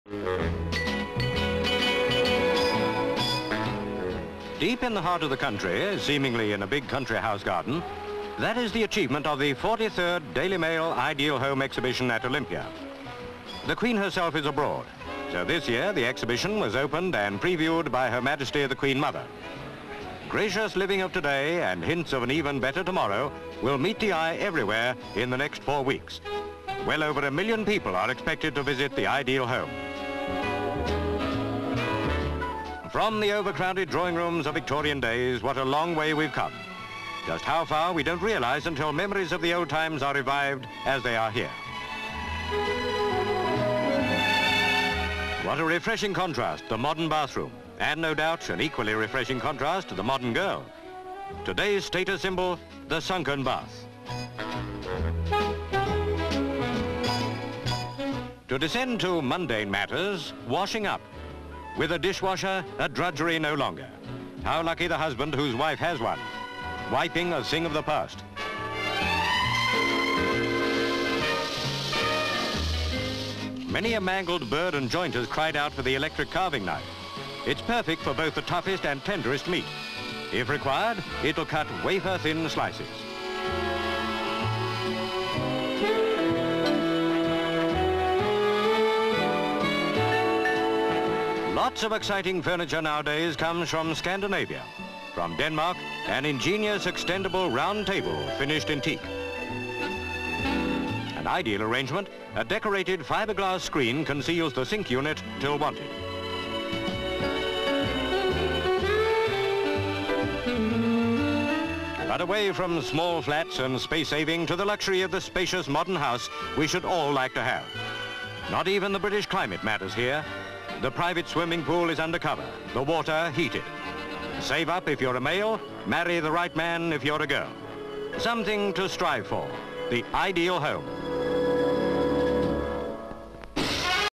Looking To Tomorrow (1966)
Olympia, London.

GV. Waterfalls and pool in garden at the Ideal Home Exhibition at London's Olympia. SV. Pan to close up shot of the flowers. SV. Queen Mother being shown around the exhibition. GV. Interior of ultra modern room with circular furniture. SCU. Child sitting in circular baby chair. SV. Woman sitting at table which is of a petal type design. SV. Elderly couple in Victorian style room. SCU. Man seated in chair. SV. Victorian bathroom. CU. Wash basin. CU. Toilet. SV. Modern day bathroom. SV. Toilet and wash basin. SV. Girl in sunken bath. SV. Woman putting crockery into dishwasher. CU. Woman closes door. CU. Dishes being washed. SCU. Electric carving knife being demonstrated, it cuts roast turkey. SCU. Queen Mother. SV. Women opening extendable Danish dining table. SV. Man and woman at breakfast table. The woman walks over and raises a partition to reveal the sink unit. She starts to wash up. GV. Looking across gardens to modern house. SV. Interior of dining room. GV. Interior showing lounge. SV. Indoor/outdoor swimming pool, pan to the lounge. SV. Roof and glass walls closing over the swimming pool. GV. The swimming pool.
 FILM ID:1783.28